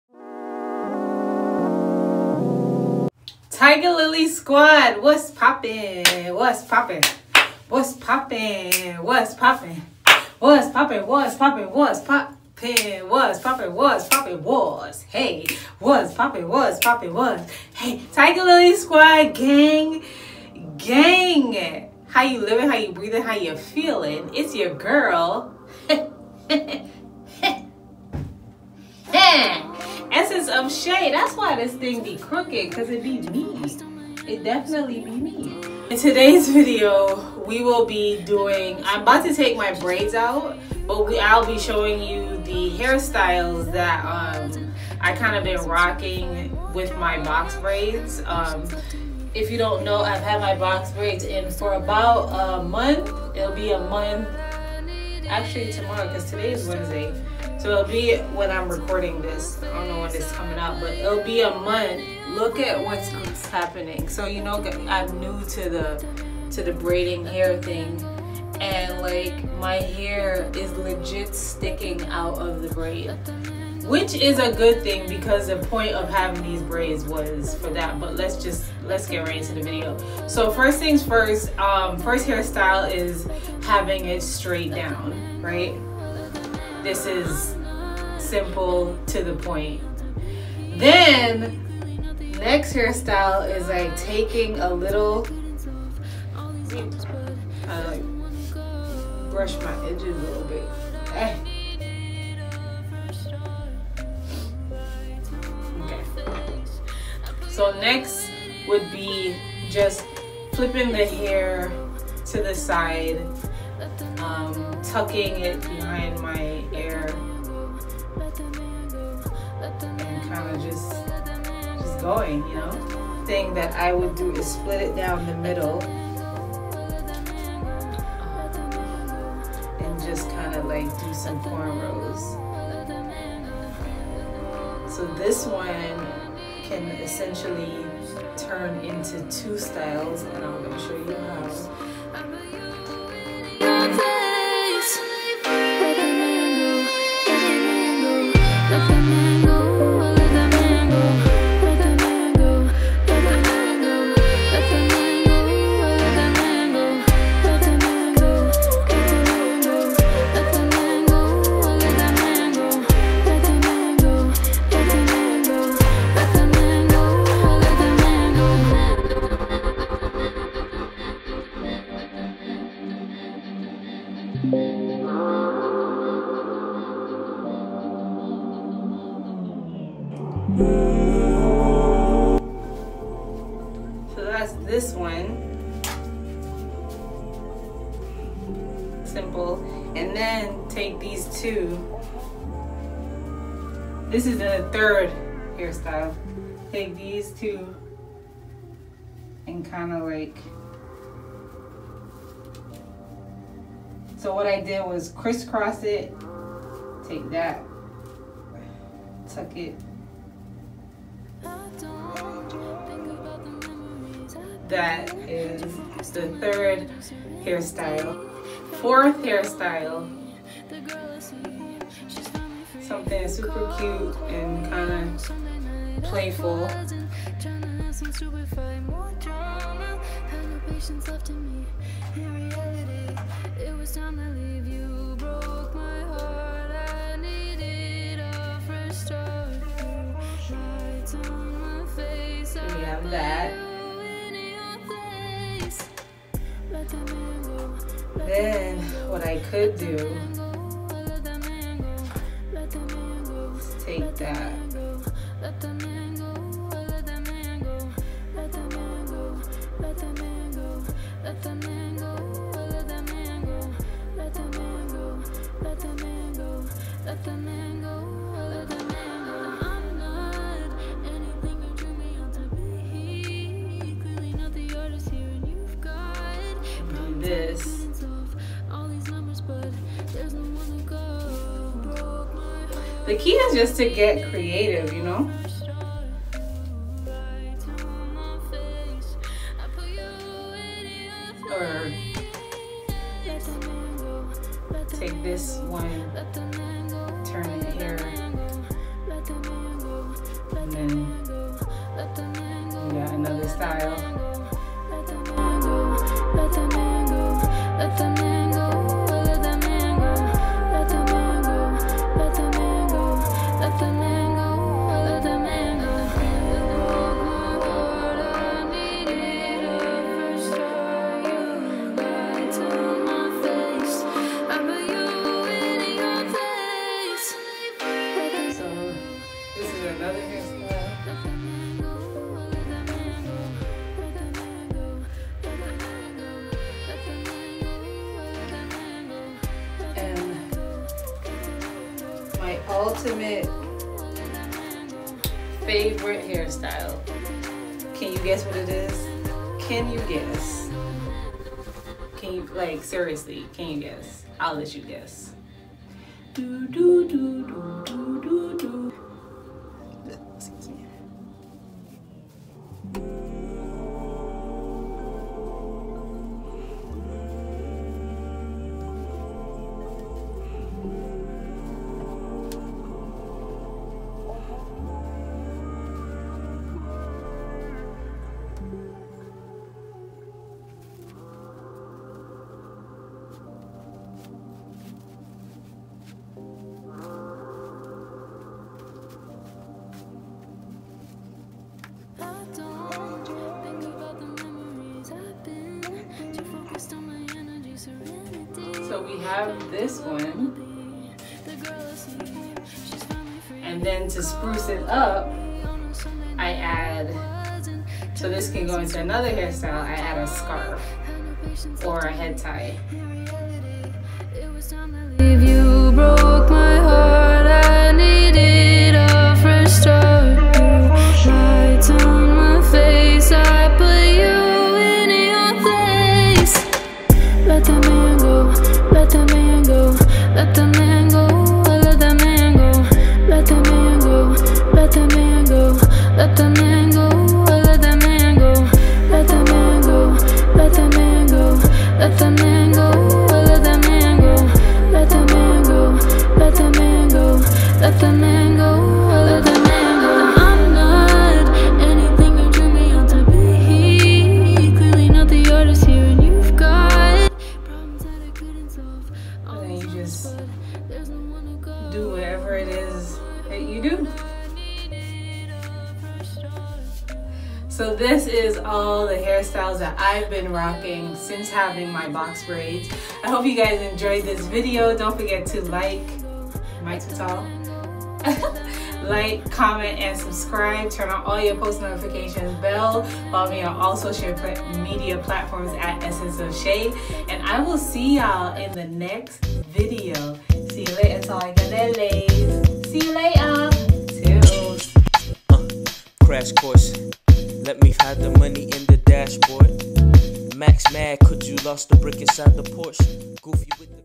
how to 10 BOX BRAIDS STYLES I ESSENCEOFSHAY
How to 10 box braids styles -

HI loves! Here are a 10 ways that I like to style my box braids. I hope that you all enjoy the video and that you all learned something from this. Hopefully it gave you some inspiration, if you're interested in getting braids!

if there is one thing I love about having braids is that, it can give me an instant face lift. be sure to not pull on them too hard, it could possibly thin out your edges. but also, just be mindful of how tight you pull on them in the beginning. Love you xoxo - Shay